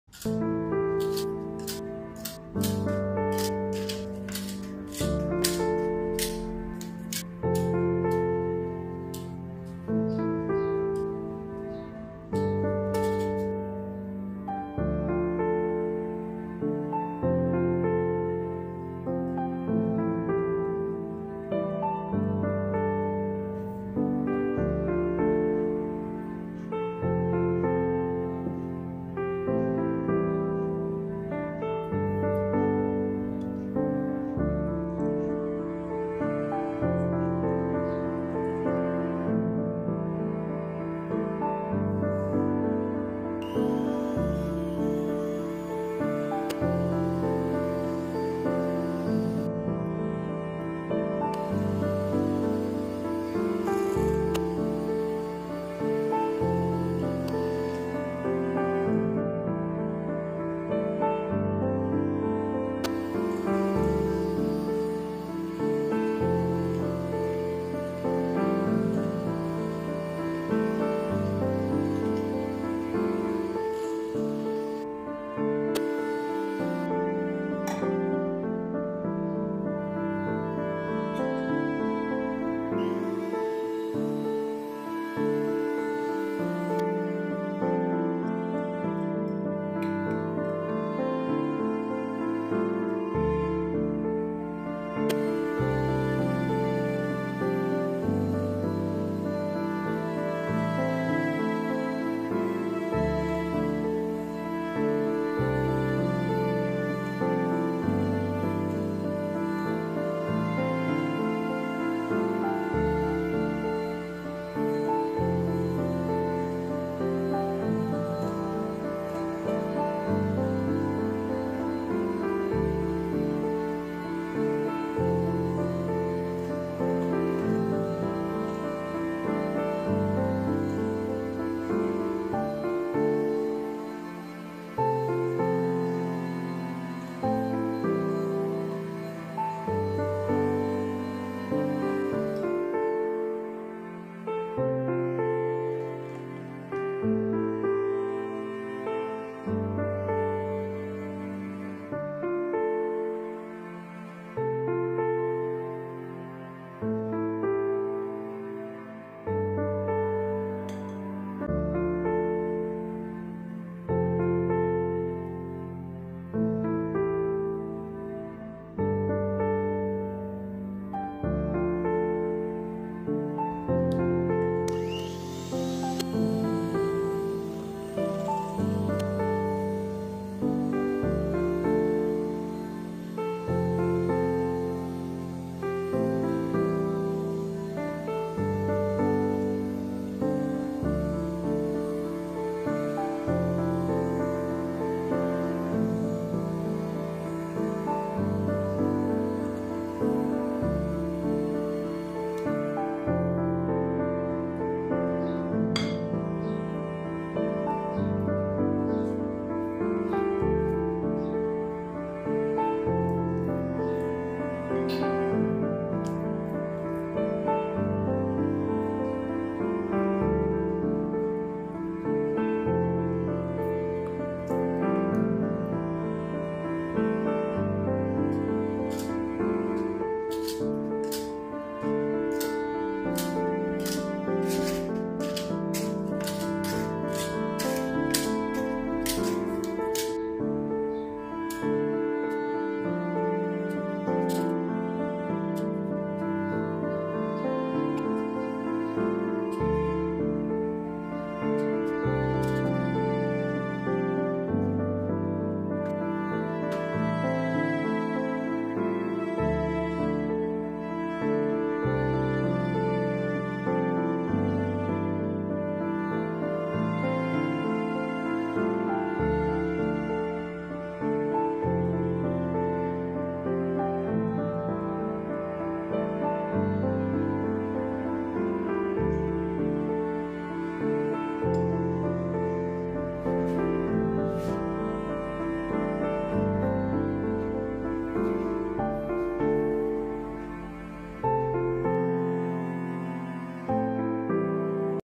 My Amateur cup cake🧁How to make Vanilla cupcake 🧁
Vanilla cupcakes

1¼ cup All-purpose flour
1¼ Tsp Baking powder
½ Tsp salt
½ cup unsalted butter
¾ cup granulated sugar
2 pieces egg
½ Tsp vanilla extract
½ cup Buttermilk
¼ cup vegetable oil